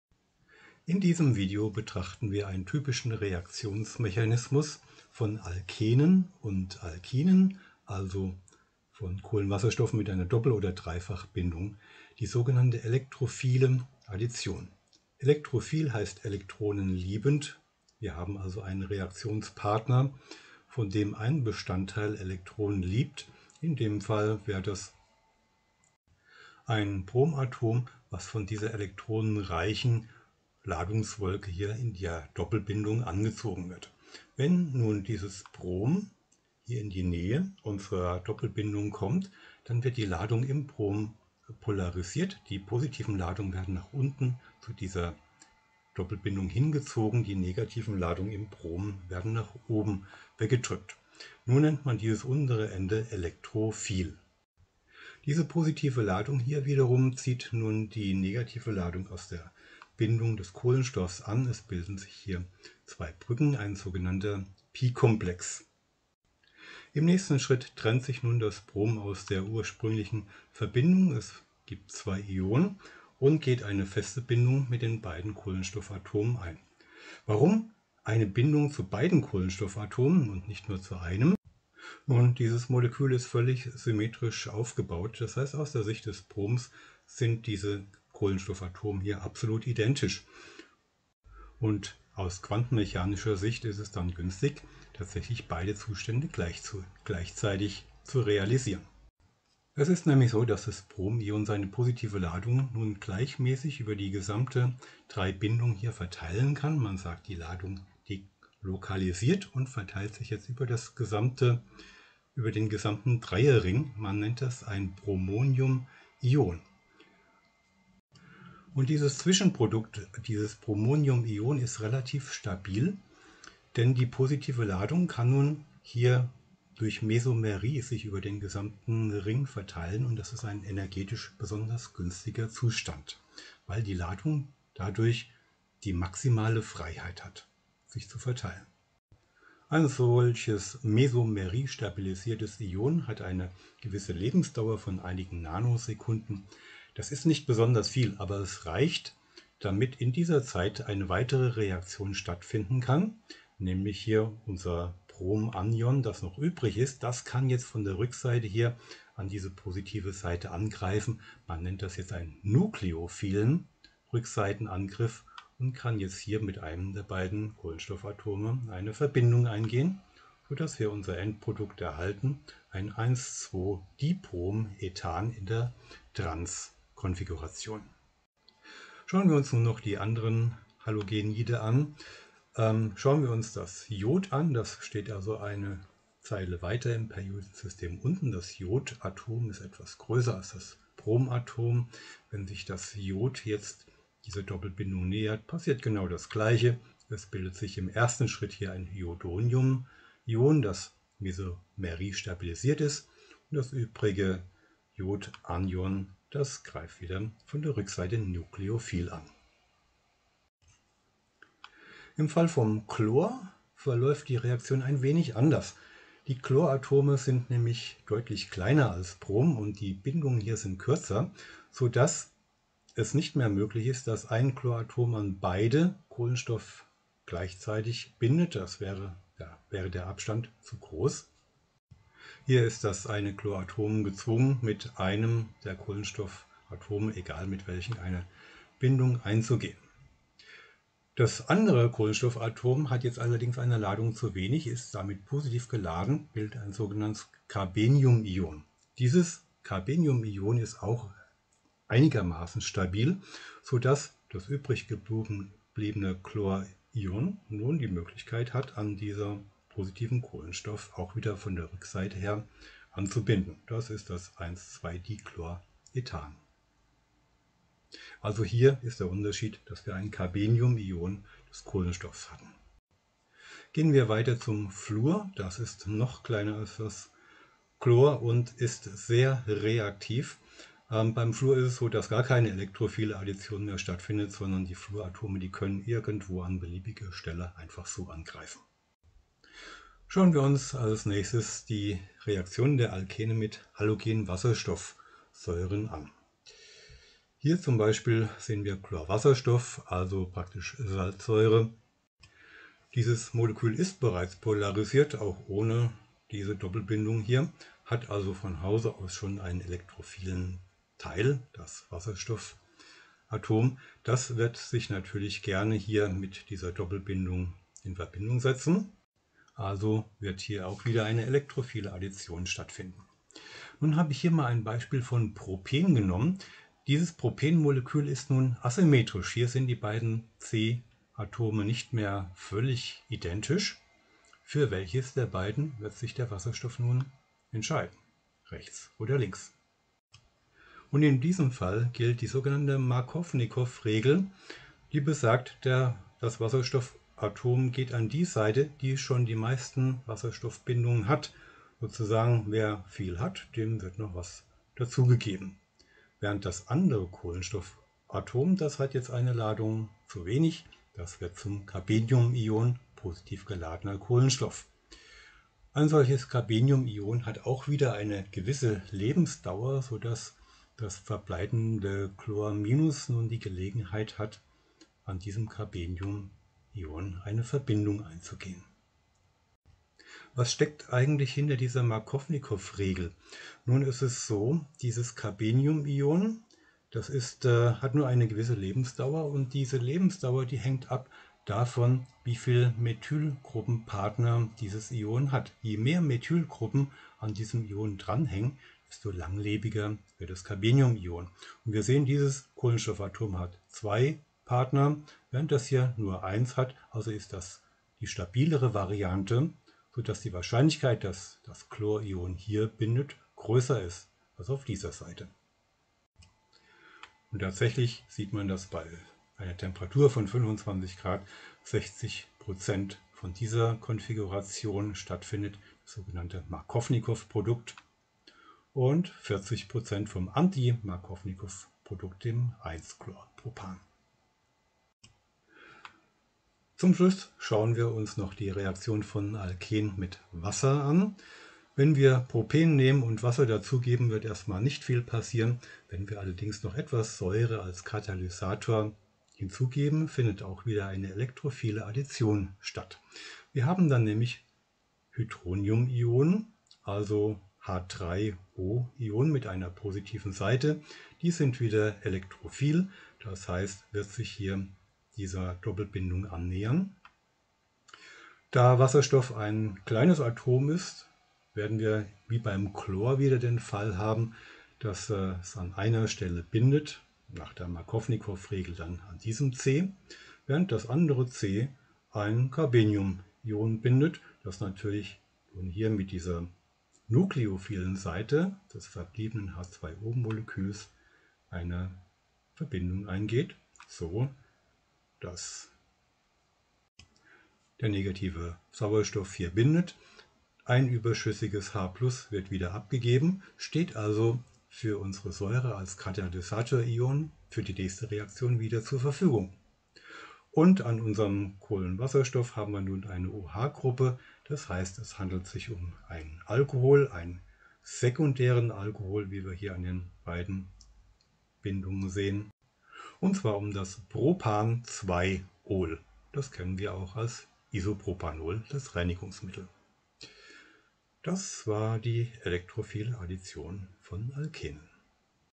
Elektrophile Addition
In diesem Video wird die Elektrophile Addition als typischer Reaktionsmechanismus von Alkenen und Alkinen in seinen Teilschritten beschrieben. Als Beispiele werden die Reaktionen mit den Halogenen Brom, Iod und Chlor gezeigt sowie mit Halogenwasserstoffsäuren und mit Wasser mit einem Säurekatalysator.

Nach diesem Video empfehle ich dir, dein Wissen in einem interaktiven Quiz zu testen:

Viel Spaß und ich freue mich auf dein Feedback! Lass mir einen Abo-Klick da.